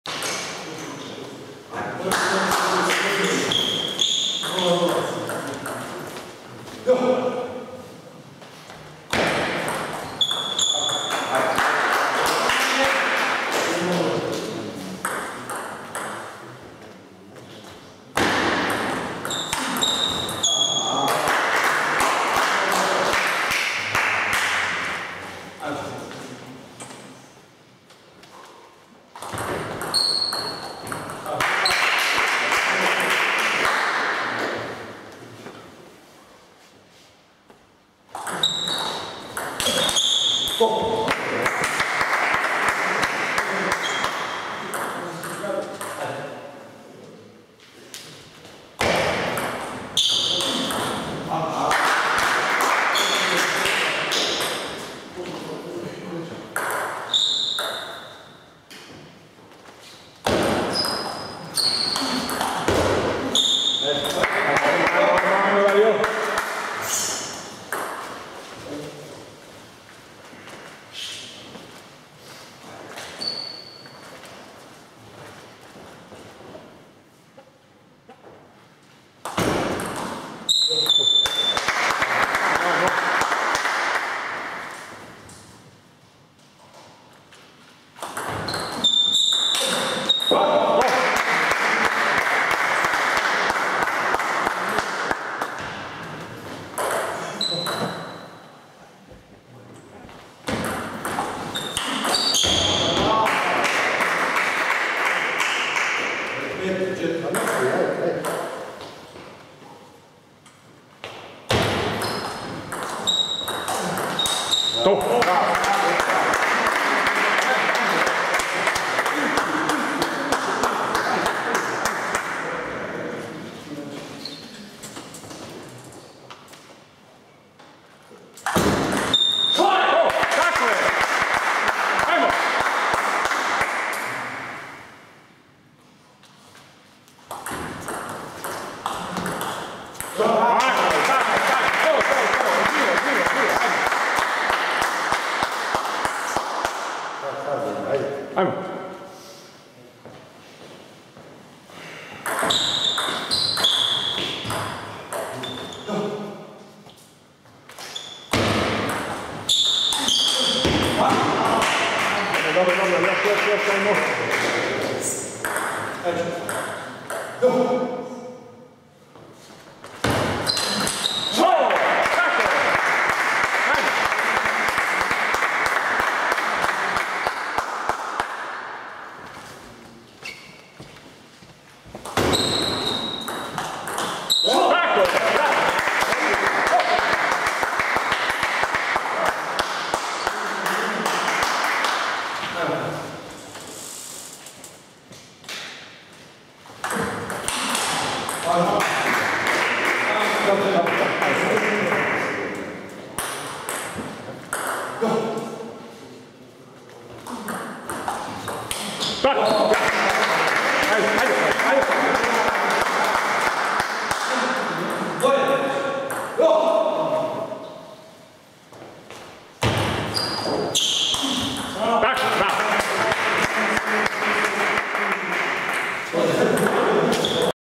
Kozić Marijan – Jurišić Dario 3 : 1 (-7, 2, 7, 8), 1. set
Opatija 08 - Brovinje 3 : 4

Kozić – Habijanec 0 : 3 (-5, -4, -5)
Kinkela – Jurišić 3 : 1 (-12, 12, 2, 8)
Salma – Ivinić 3 : 0 (4, 9, 7)
Kozić, Kinkela – Dobrić, Habijanec 1 : 3 (-8, 3, -7, -3)
Kozić – Jurišić 3 : 1 (-7, 2, 7, 8)
Salma – Habijanec 1 : 3 (-6, -6, 9, -6)
Kinkela – Dobrić 1 : 3 (-9, 4, -7, -5)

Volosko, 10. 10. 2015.